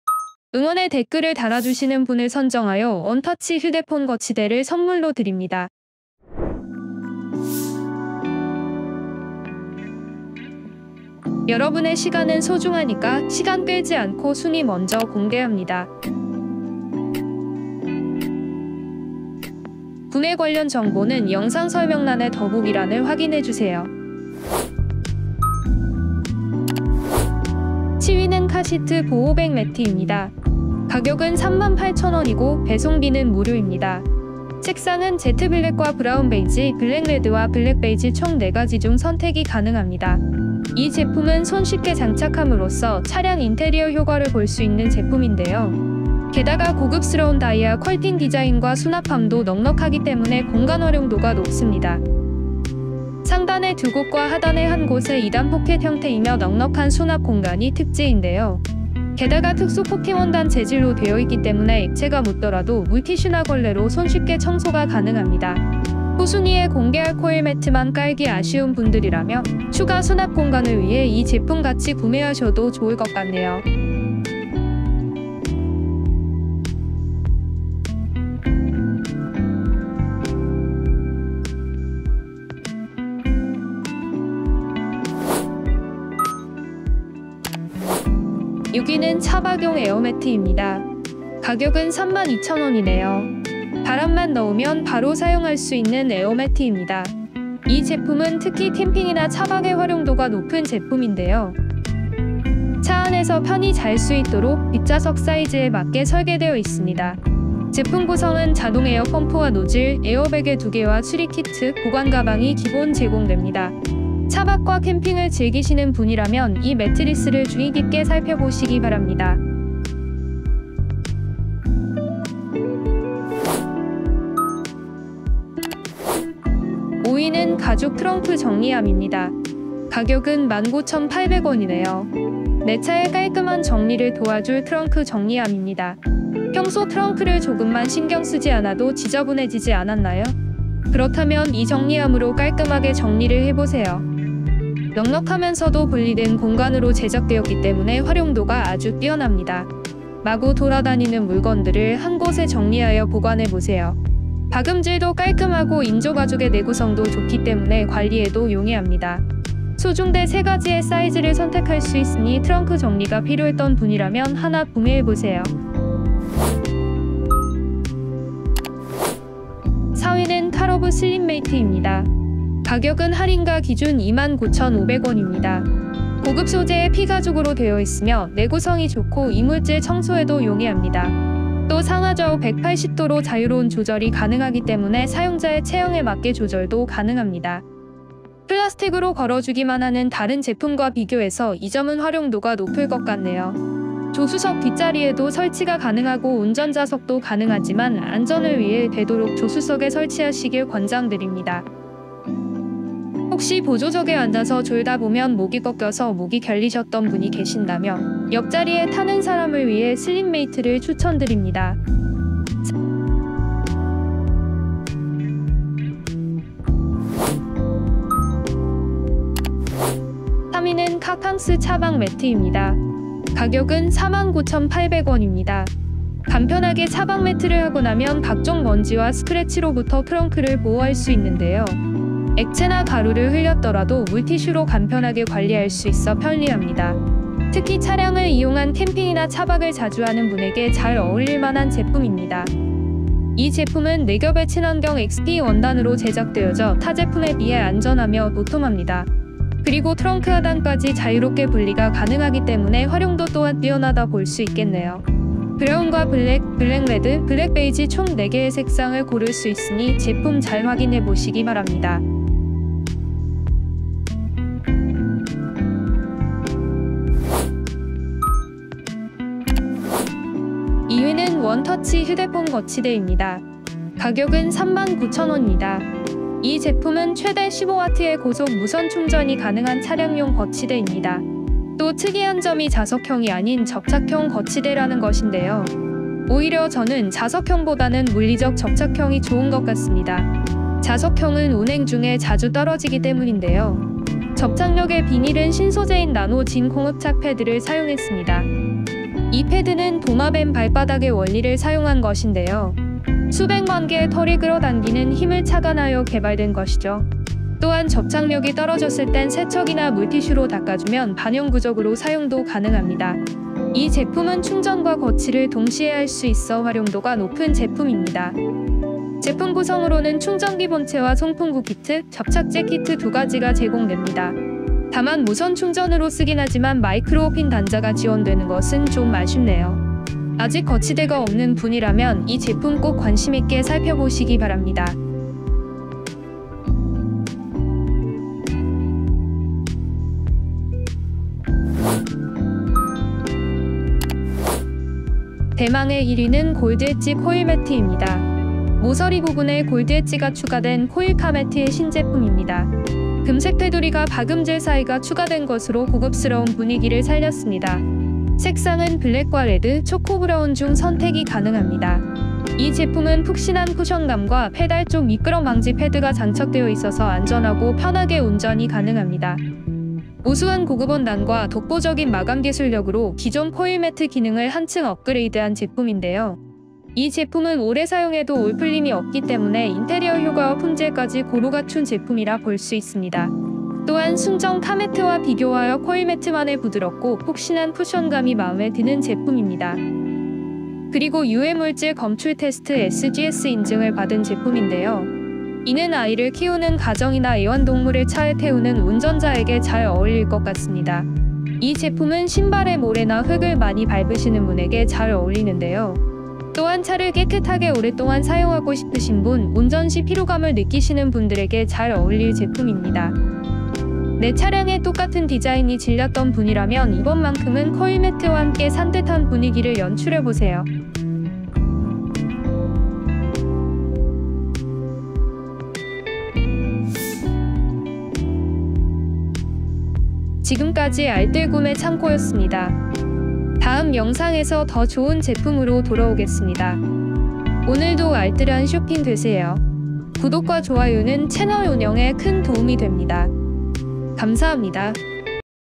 내 차를 고급지게 업그레이드 해 줄 가성비 자동차용품 BEST 7 [훕몰]
*질문&비지니스 문의

*본 영상은 제품 및 제작비를 지원 받았습니다.
*이벤트 또한 포함되어 있으니 무료 증정 이벤트에 참여 해 보세요.

가성비 자동차용품 BEST 7 [0:00]

7위 카시트 보호백매트 / 38,000원 [0:20]

6위 차박 매직에어매트 / 32,000원 [1:26]

5위 가죽 트렁크 정리함 / 19,800원 [2:04]

4위 카러브 슬립메이트 / 29,500원 [2:48]

3위 카캉스 차박매트 / 49,800원 [3:39]

2위 원터치 휴대폰거치대 / 39,000원 [04:28]

1위 골드엣지 코일매트 / 138,000원 [5:43]

오늘 하루도 즐거운 알뜰 구매 쇼핑 되시기를 바랍니다 :D
본 영상은 알뜰구매창고에서 직접 선정한 순위 영상이며 주관적인 생각이 포함되어 있습니다.